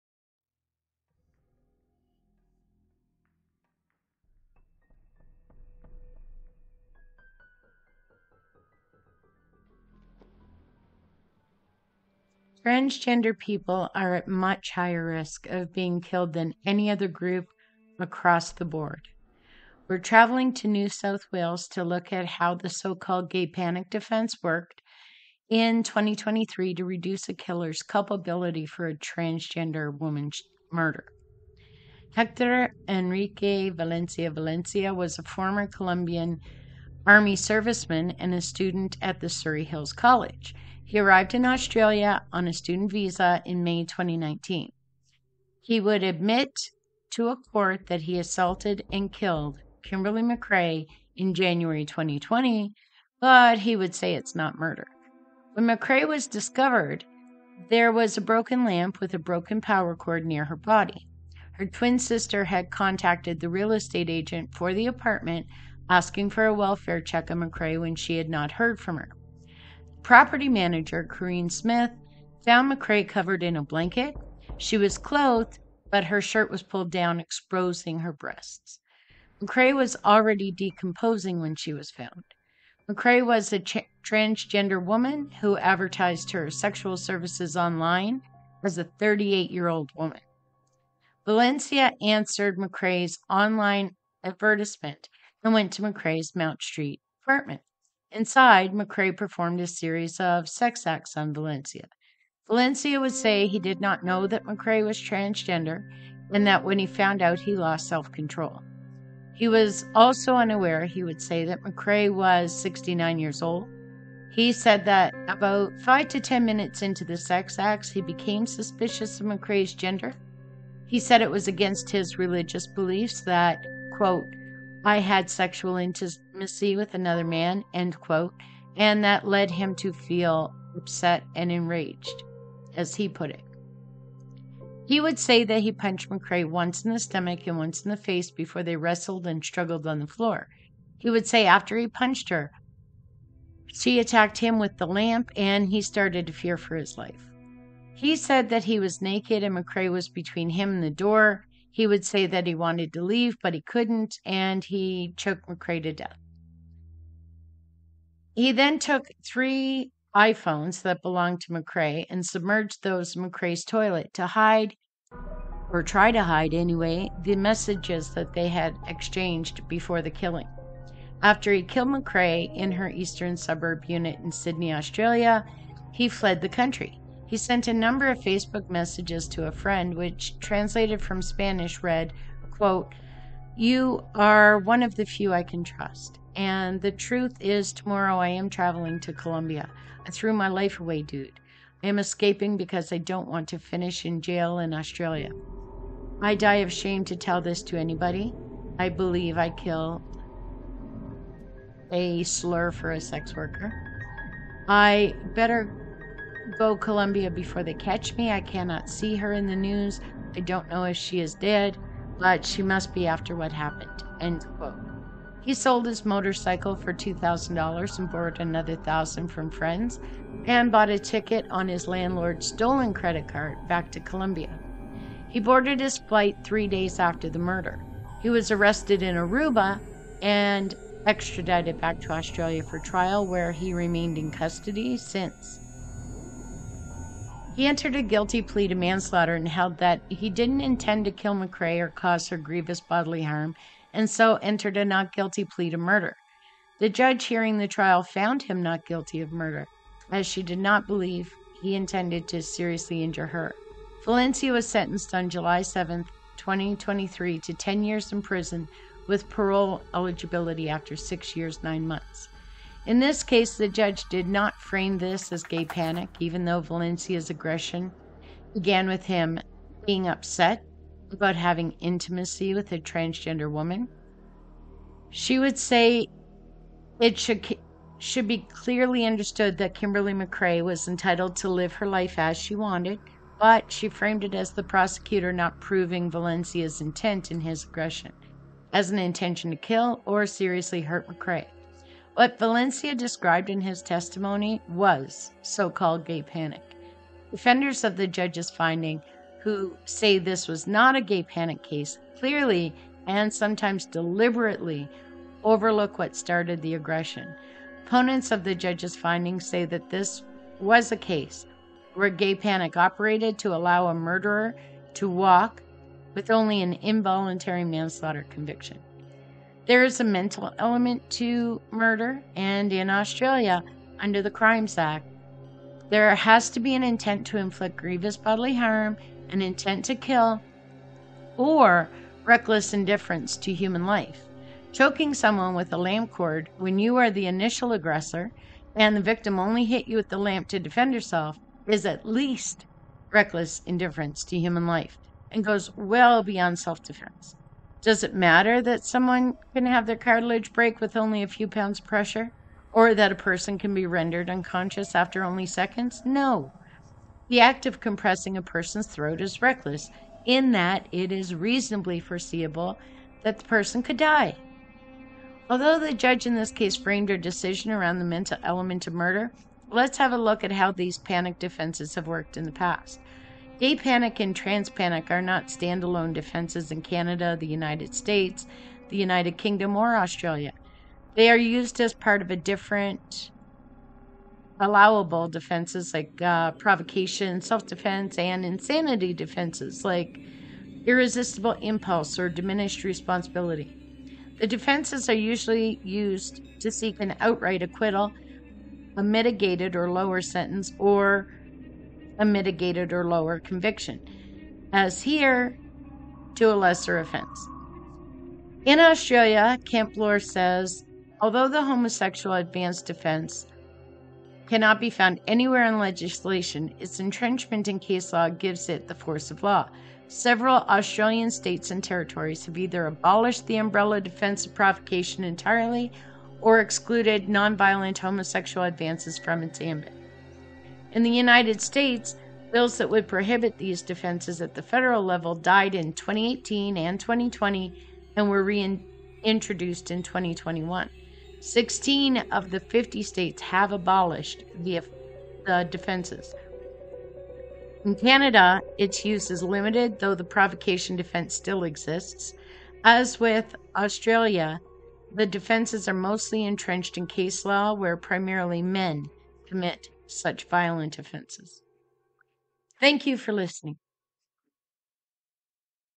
The Tragic Murder of Kimberley McRae
He killed her for being who she was.